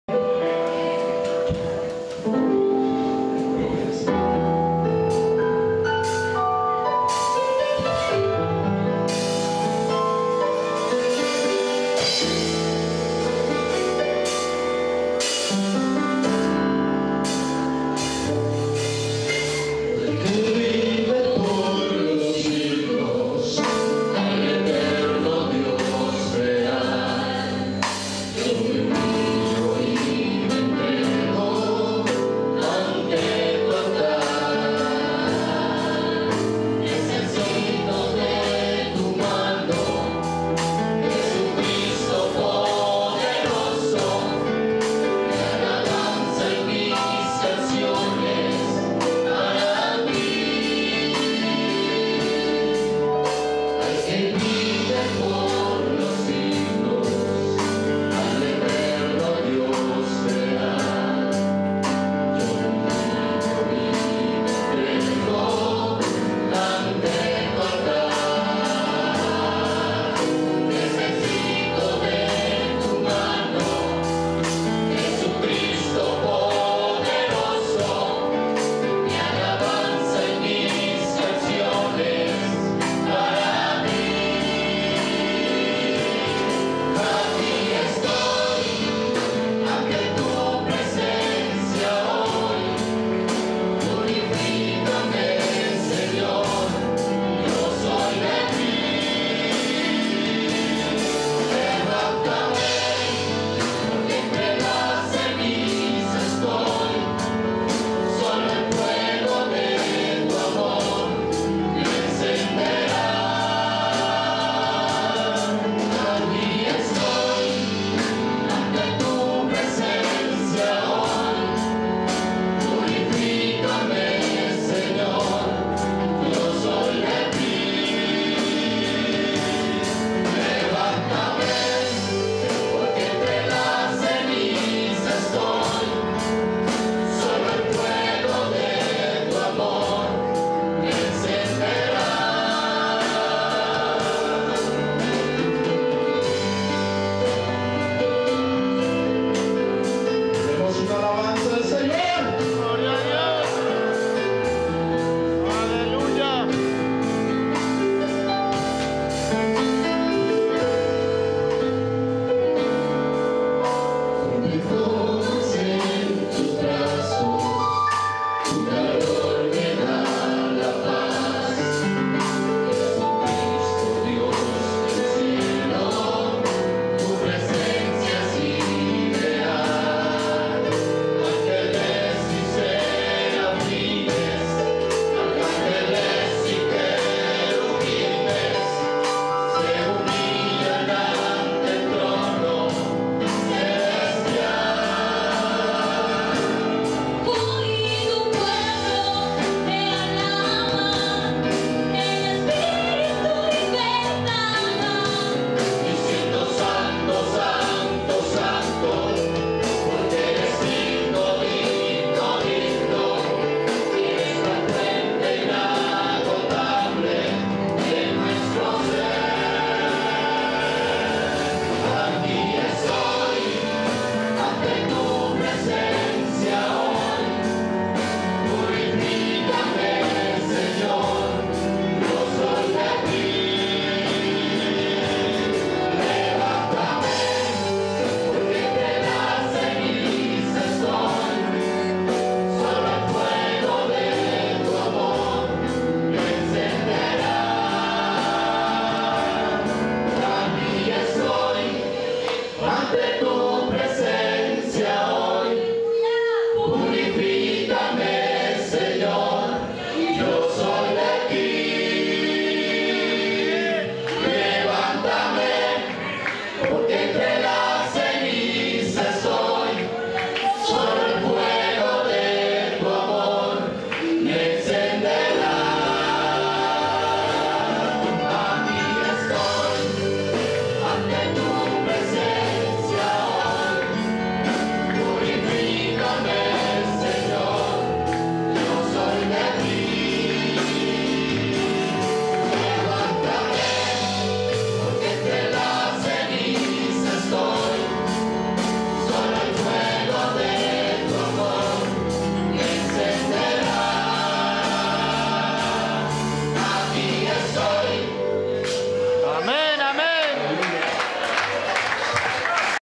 grupo de alabanza Saraverna
Grupo de alabanza en Saravena-central
 primer congreso regional de música-Agosto 2012